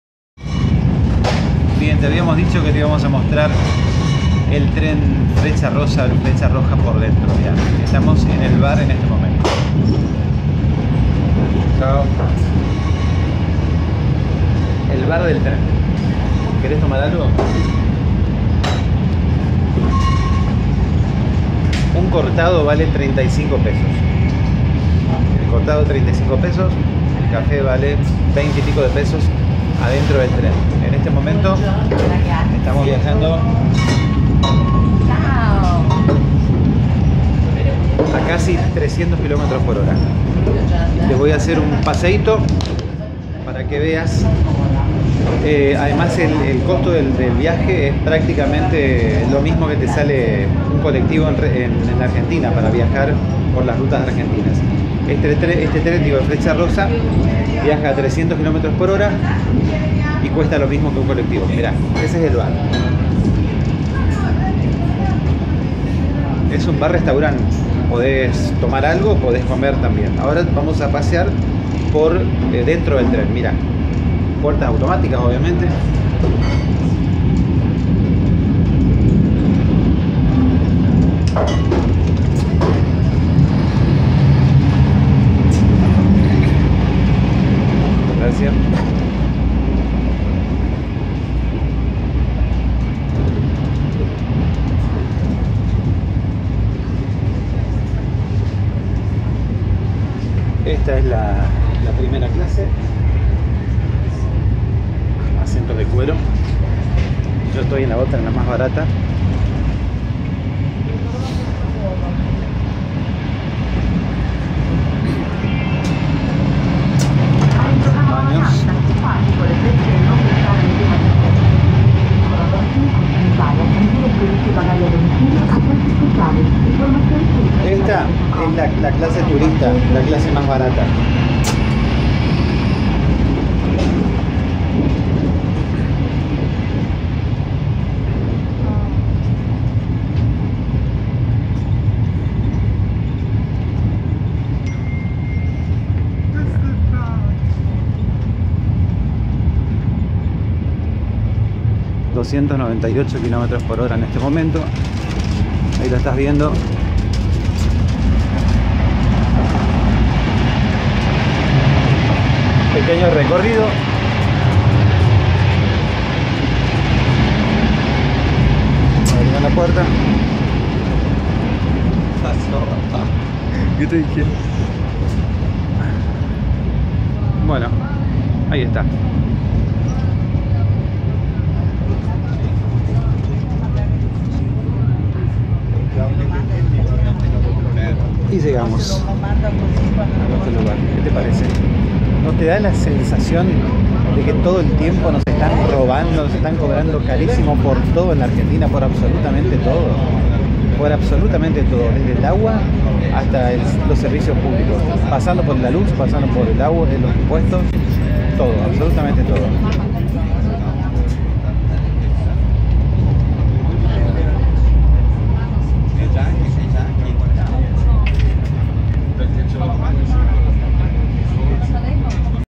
Viajamos a 300 kmts por hora en tren, te invito a conocerlo por dentro !
El Freccia Rossa es el tren más veloz de Italia. Viaja a 300 kms por hora. El pasaje, sacado con anticipación,  cuesta alrededor de $3 por kilómetro, los menores de 15 años viajan gratis. Tiene Restaurante y baños que son un lujo.

Seguime también en

EL PODER CIUDADANO